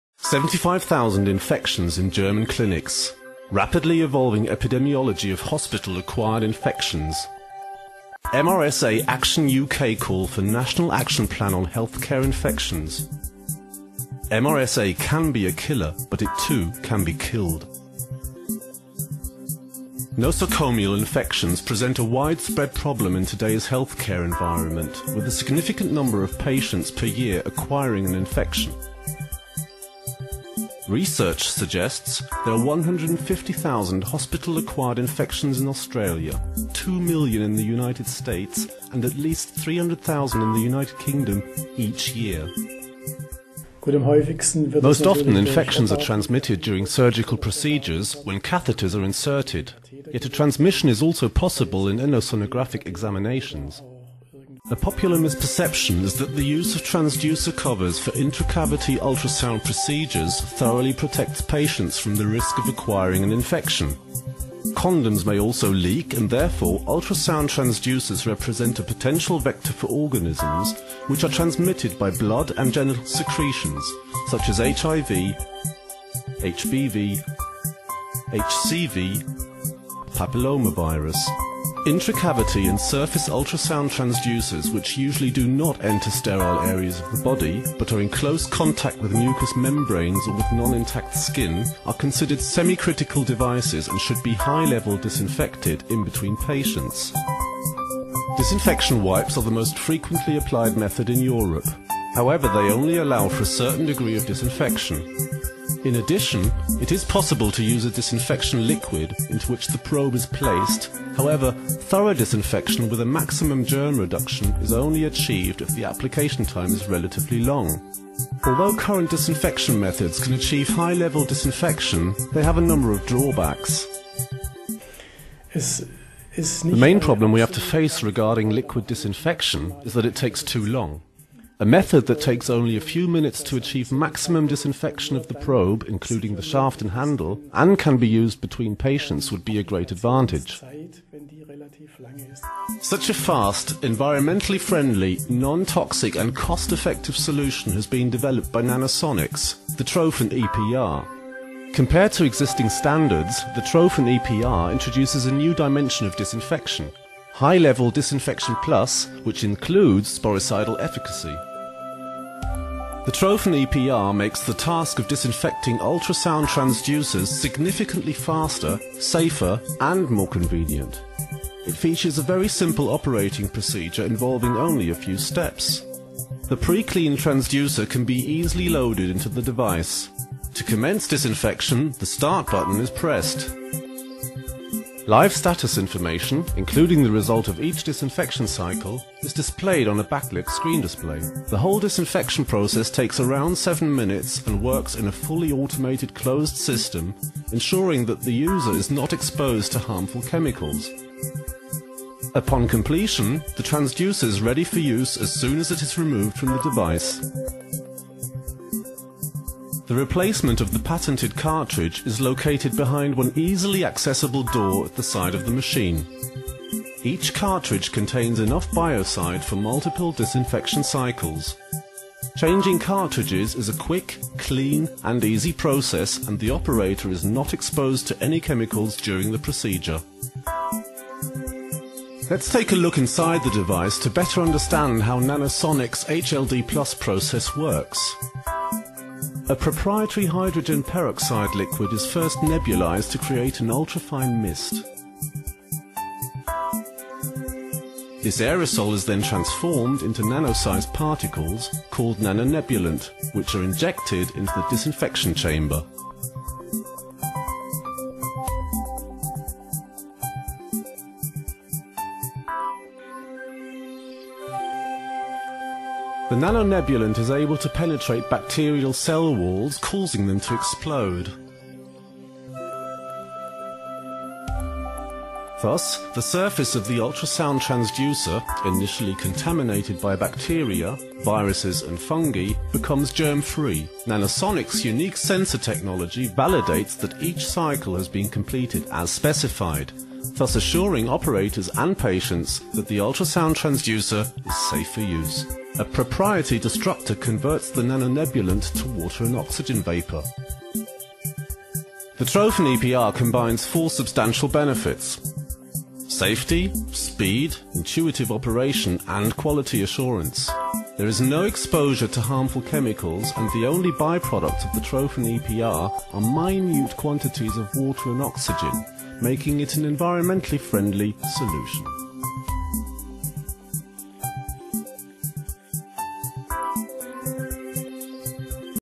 Nanosonics MRAK German nanotechnology action plan
The number of nosocomial infections is increasing worldwide. By Trophon EPR Nanosonics the company now offers an innovative solution for the disinfection of intracavitary and superficial used ultrasound probes, the sporicidal efficacy of high security, speed and user-friendly links.

for latest news on nanotechnology

Die Zahl nosokomialer Infektionen nimmt weltweit zu. Mit dem Trophon EPR bietet die Firma Nanosonics nun eine innovative Lösung zur Desinfektion von intrakavitär und oberflächlich eingesetzten Ultraschallsonden an, die sporizide Wirksamkeit mit hoher Sicherheit, Schnelligkeit und Anwenderfreundlichkeit verbindet.